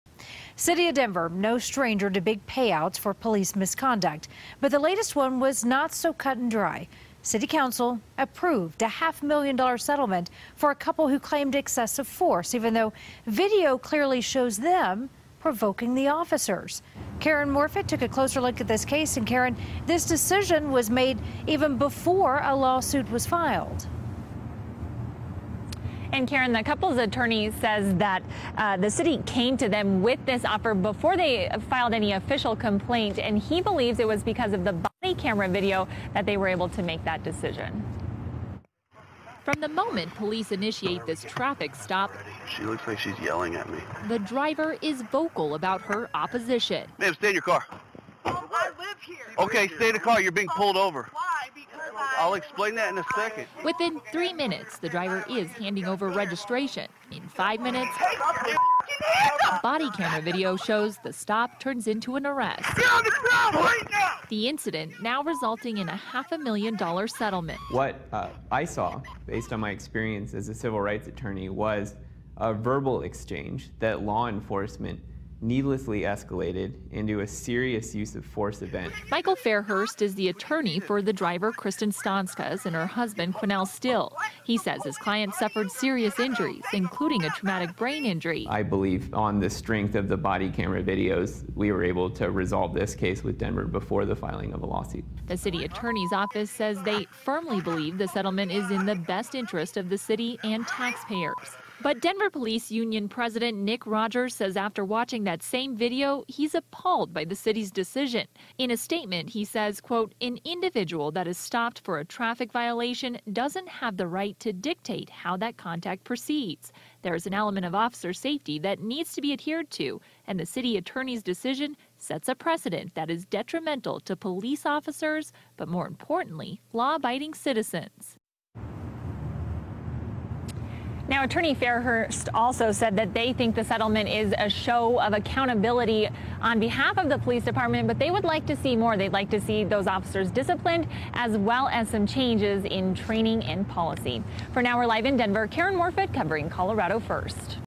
Controversial & 'Detrimental' Settlement Costs Denver Taxpayers $500,000
The City of Denver is paying half of a million dollars of taxpayer money to a couple.